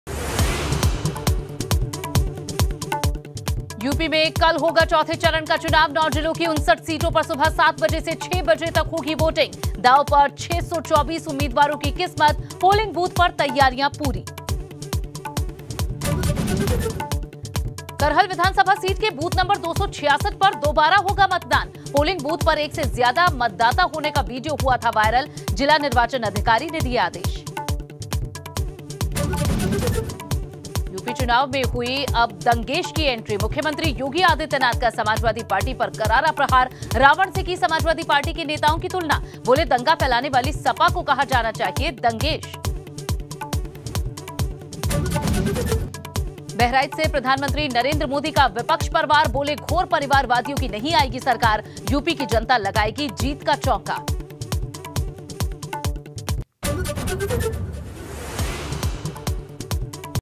Headline : UP में कल होगा चौथे चरण का चुनाव, दांव पर 624 उम्मीदवारों की किस्मत ! | Hindi News
यूपी में कल होगा चौथे चरण का चुनाव 9 जिलों की 59 सीटों पर सुबह 7 बजे से शाम 6 बजे तक होगी वोटिंग...दांव पर 624 उम्मीदवारों की किस्मत .पोलिंग बूथ पर तैयारियां पूरी